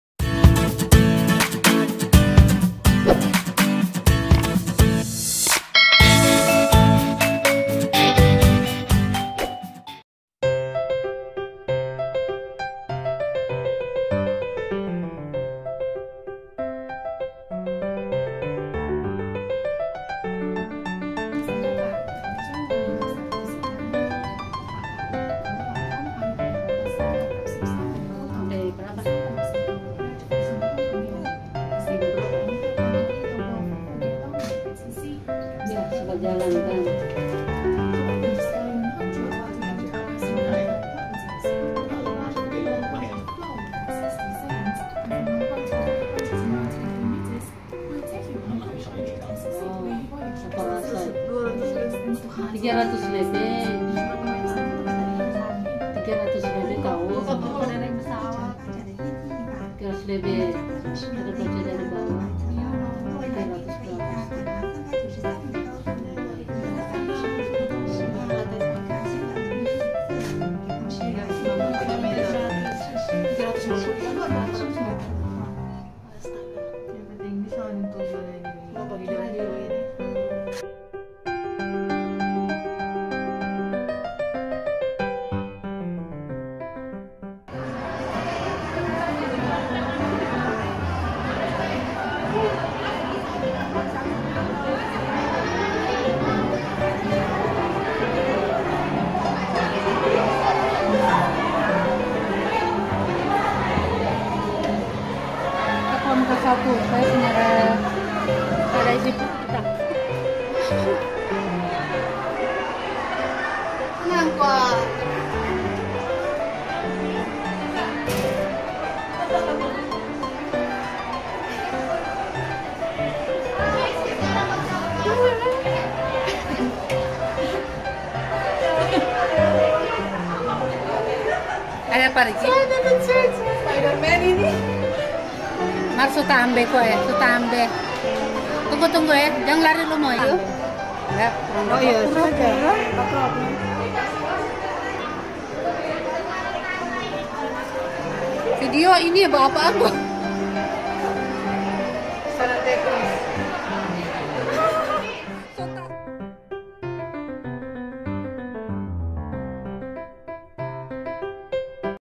view beautyfull hongkong
Sky100 adalah gedung ke 2 tertinggi di hongkong
Berlokasi di kowloon
Cara kesana, naik mtr exit C2 trus ikuti petunjuk
MTR Austin exit b juga bisa ikuti petunjuk